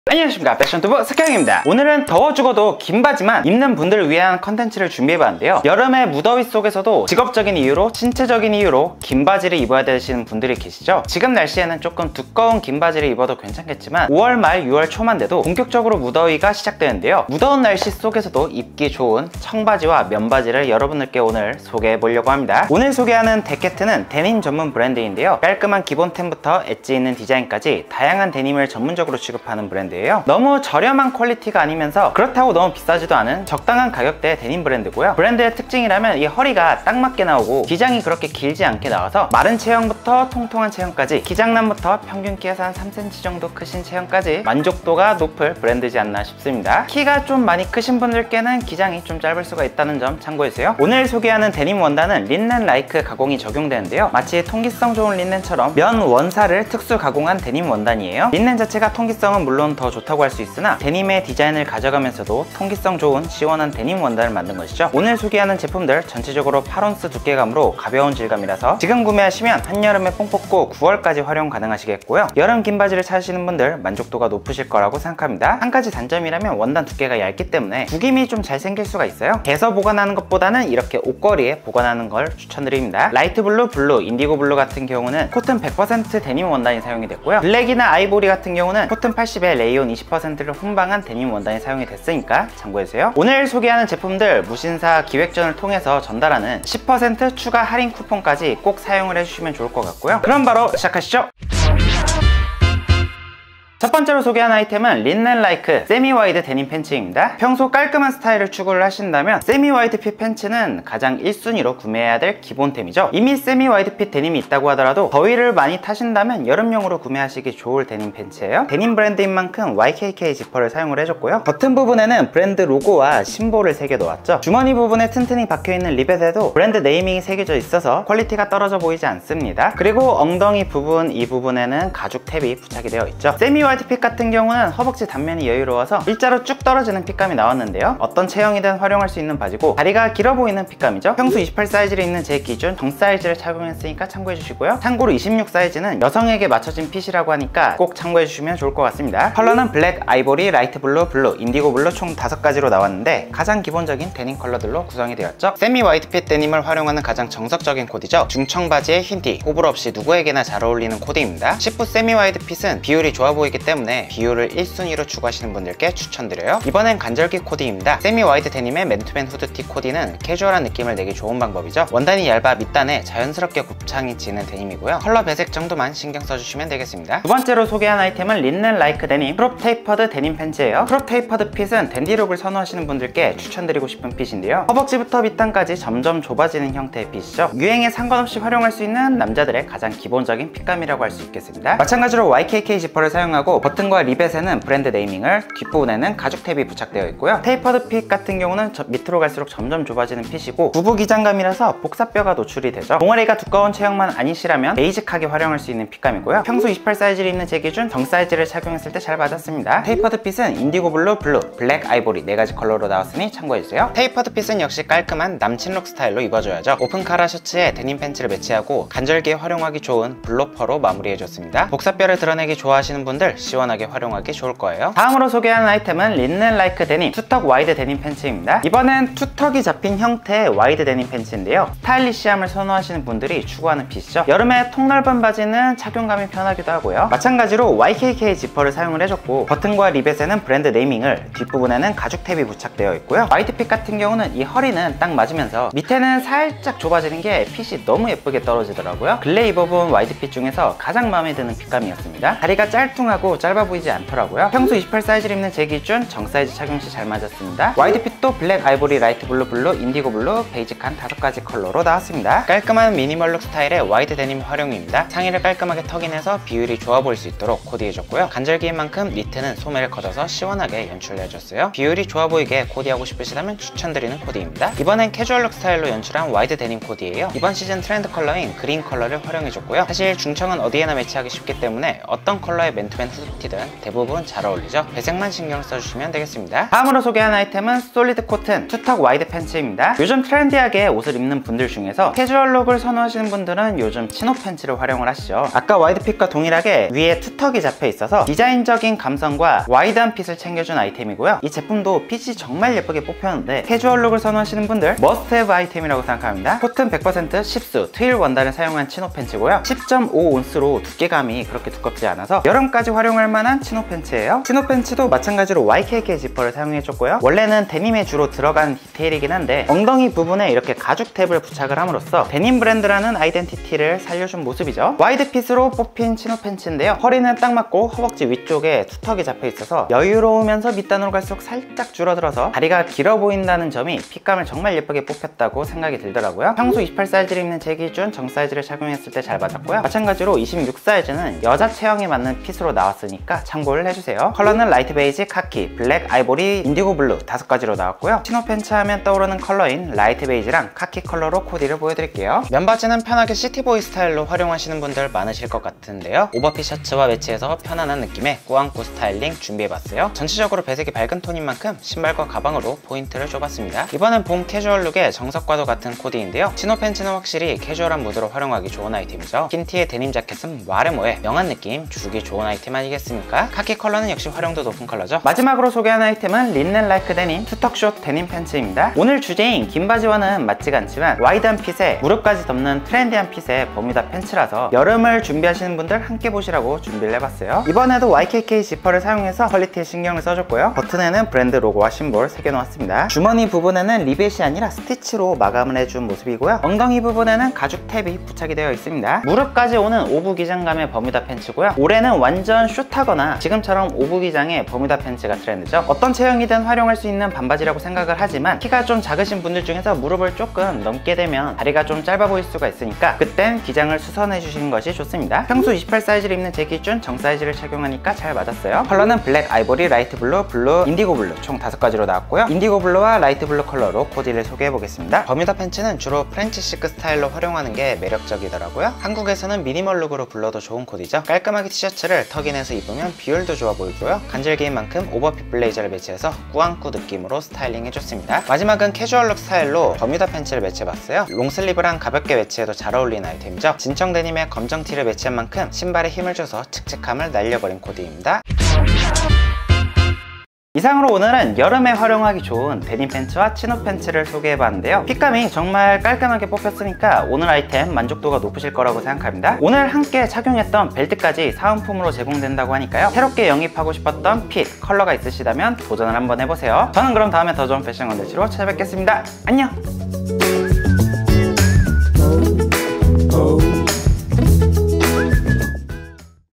더워 죽어도 긴바지라면! 여름 청바지&면바지 추천
더워 죽어도 긴바지만 찾는 분들 계시죠! 오늘은 여름에 활용하기 좋은 데님 팬츠와 치노 팬츠를 소개합니다 😉

삭형 Spec.
166cm 62kg
상의 : 95
하의 : 28
신발 : 250

00:00 intro
00:22 DECET 브랜드 소개
00:50 여름 원단 소개
01:42 [린넨라이크데님] 세미 와이드 데님 팬츠
02:57 [린넨라이크데님] 크롭 테이퍼드 데님 팬츠
03:45 [린넨라이크데님] 투턱 와이드 데님 팬츠
04:47 솔리드 코튼 투 턱 와이드 팬츠
06:16 [린넨라이크데님] 투턱 숏 데님 팬츠
07:29 마무리

EDIT BY 고종찬
촬영장비 : 소니 A7S3 / A7M3 / A6400